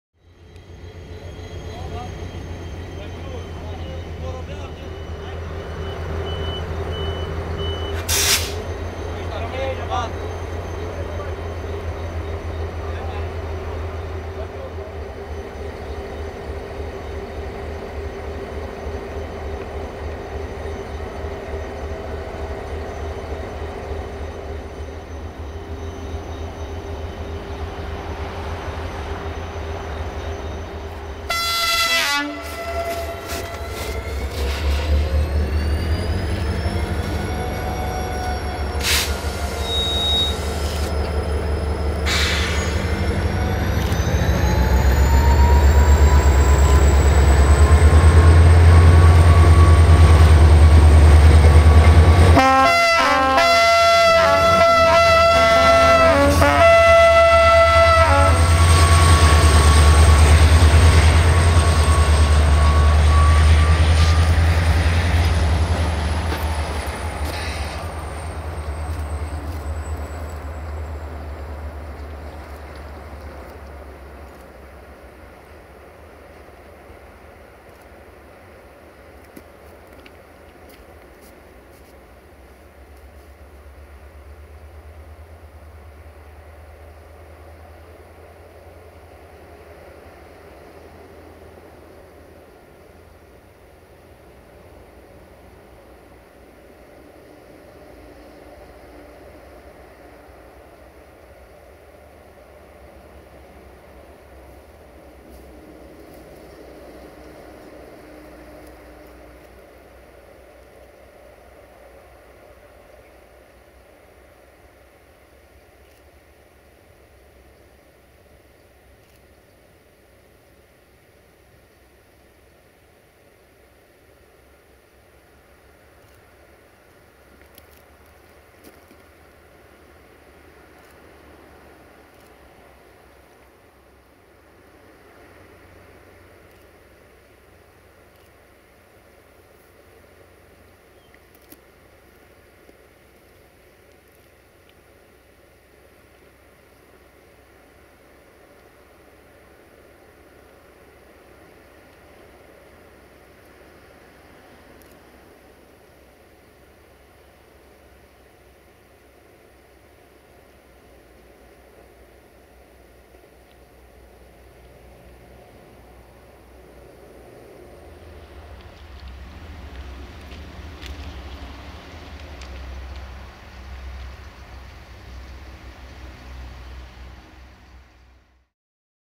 Jimmy 65-1300-6 Locomotiva de Ajutor Spre/Helpful Loko To Poieni [Winter Edition] - 25 January 2017
1: Regina Bihorului~Jimmy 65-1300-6 RO-SNTFC Pleaca Izolata spre Poieni din Gara Piatra Craiului/Go Single to Poieni from Piatra

Craiului Station

[Winter Edition]

25 January 2017